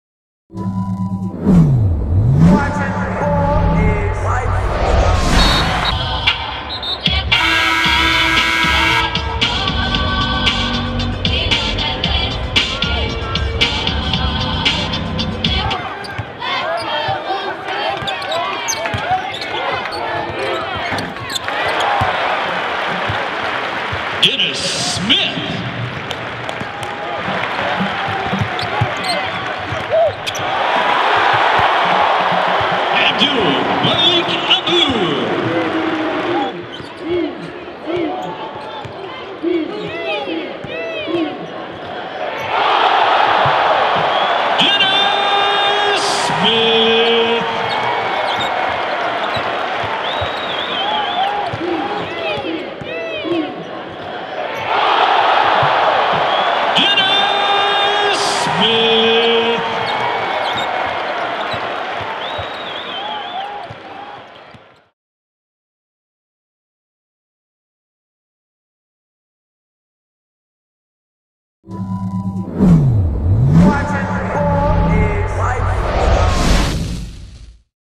Dennis Smith Catches 1st Body in Front of J Cole & Wale!
NC State won a close game against Boston University on Saturday afternoon in Raleigh's PNC Arena. Dennis Smith Jr scored 15 points while adding 3 assist 2 rebound and one emphatic dunk. The dunk got approval from J Cole &amp; Wale who were sitting front row for the game. Check out some of Dennis' best highlights from the game and keep it locked here for all the highlights from Raleigh all season long!